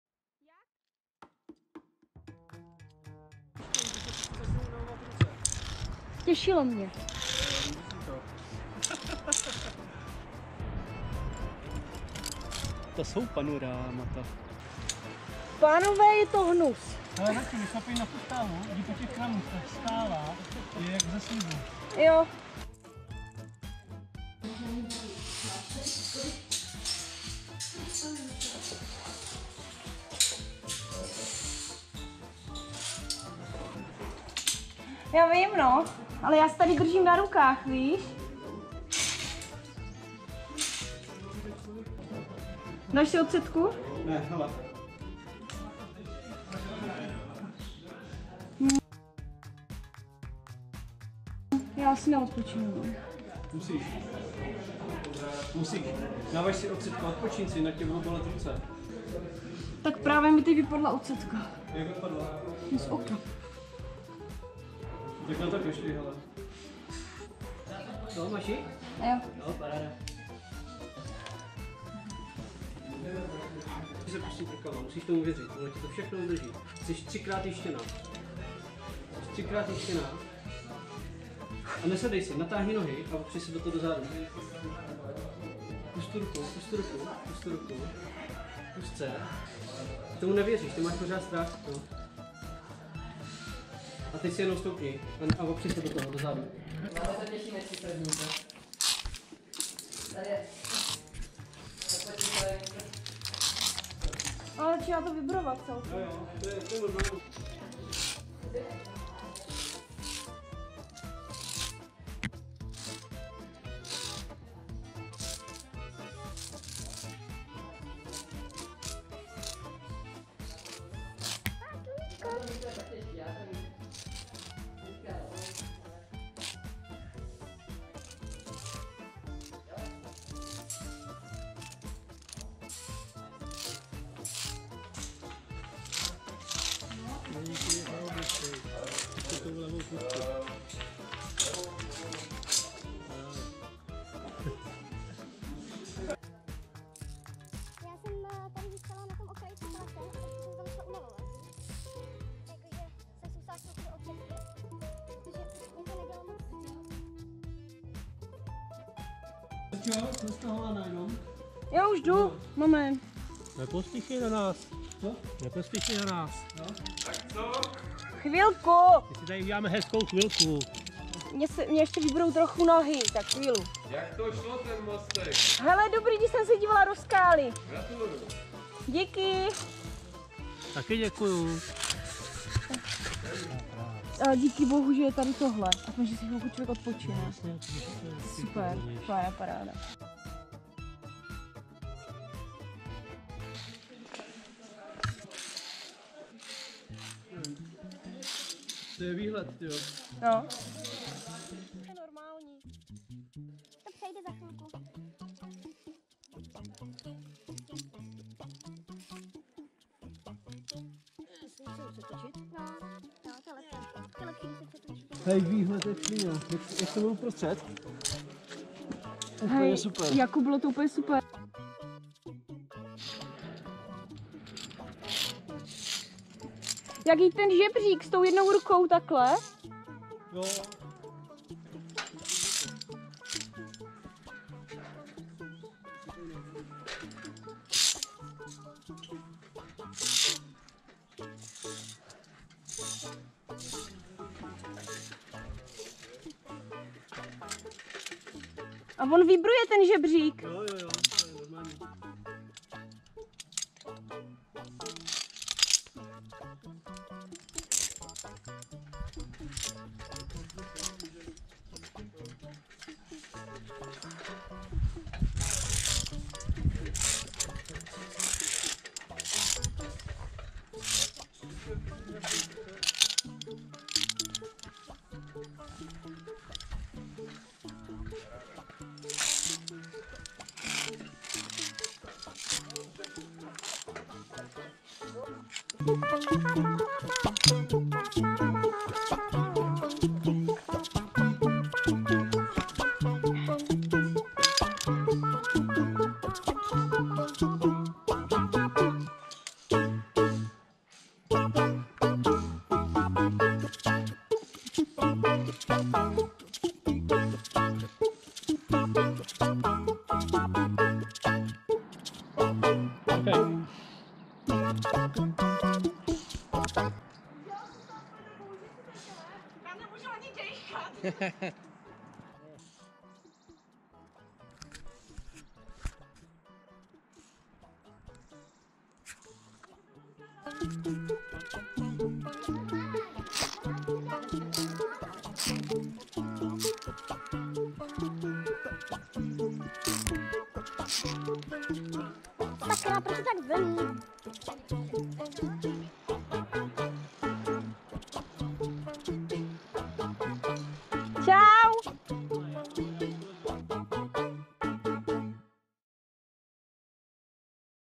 GOpro 7: TŘESOUCÍ SE KOLENA ZAČÁTEČNÍKŮ NA via FERRATA
O CO ŠLO?:
Sestřih  mostů (z ocelových lan), vibrujících žebříků a kluzkých skal X metrů nad zemí (vybrala jsem  několik zajímavějších částí).
 V zájezdu začátečníků Rakousko-Solné jeskyně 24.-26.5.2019= okolí Bad Goisern.
 Bylo nám řečeno, že jsou to nejlehčí Ferraty v Rakousku. :)
JElikož jsme někteří neměli moc s čím srovnávát, jen s pár skalami u nás, zapsal se do našich myslí tento nezapomenutelný výlet. Nejen nádhernou přírodou, ale výškami ve kterých jsme se mnozí ocitli po prvé. Na záběrech vidíte např. zdolání dvou mostů asi 10 metrů dlouhých. Na ferratách Mein Land Dein Land a Laserer Alpin- obě s obtížností (C). Poslední ferrata Laserer Alpin nad jezerem (Vorderer Gosausee) mostek měřil asi 50 metrů.
Dále jsme lezli například krásnou ferratu Leadership  Klettersteig.

Dobře se bavte. :)
Pokud se video líbilo můžete ocenit palcem ukazujícím vzhůru.